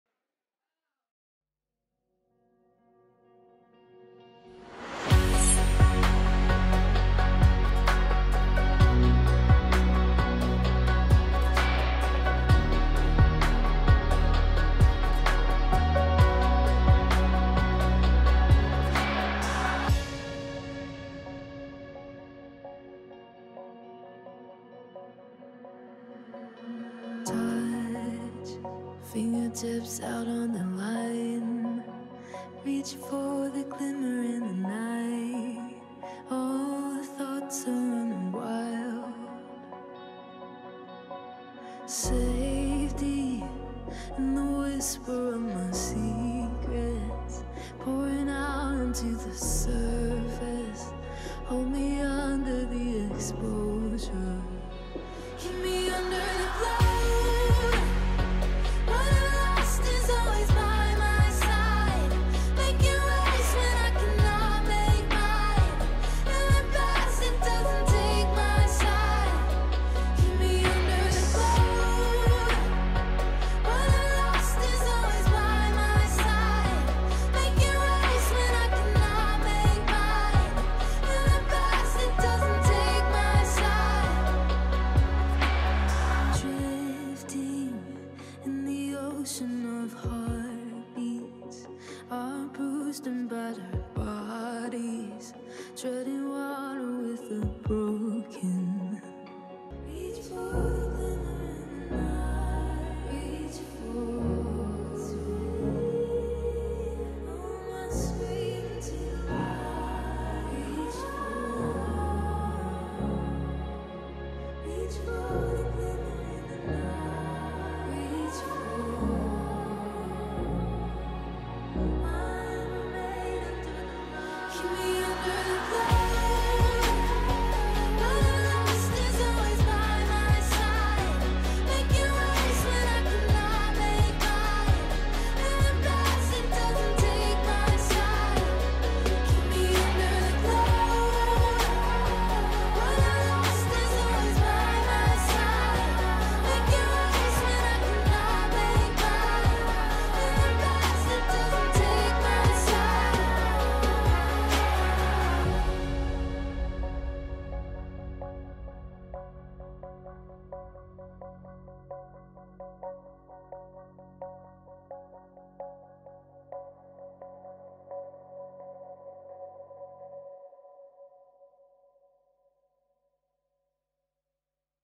Cinematic Wedding Highlights || XT4 (16-50mm WR Lens)
Rinchhan and Rebecca's Wedding 2022